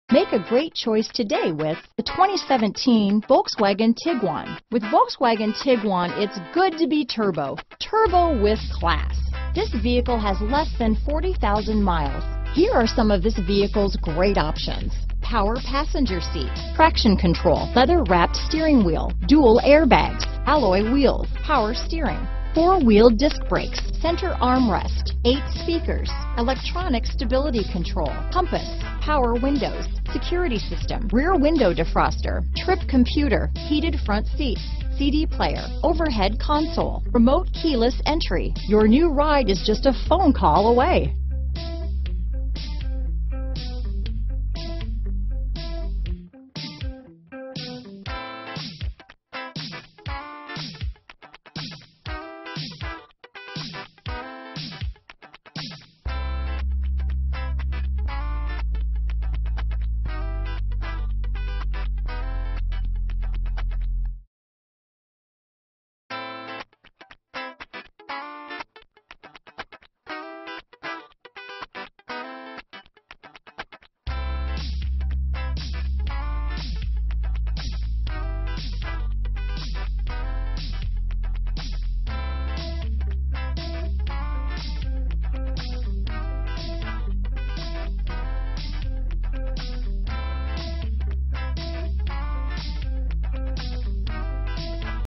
2017 Volkswagen Tiguan Tyler, Longview, Lufkin, Nacogdoches, Shreveport, TX T2780
Pacific Blue Metallic Used 2017 Volkswagen Tiguan available in Tyler, Texas at

Patterson Volkswagen
3800 Chandler Hwy

**FREE LIFETIME WARRANTY**.brbrbr2017 Volkswagen Tiguan Wolfsburg FWD 6-Speed Automatic with Tiptronic 2.0L TSI DOHC BluebrbrbrReviews:br  * Sporty handling doesn't come at the expense of a comfortable ride quality (except Sport trim); engine offers punchy turbocharged performance; interior materials feel premium; cabin is quiet at highway speeds. Source: Edmundsbr  * Cargo capacity on par with compact crossovers; upscale interior; responsive and fuel-efficient engine; sophisticated ride; standard all-wheel drive and extra ground clearance compared to the regular Golf wagon. Source: Edmunds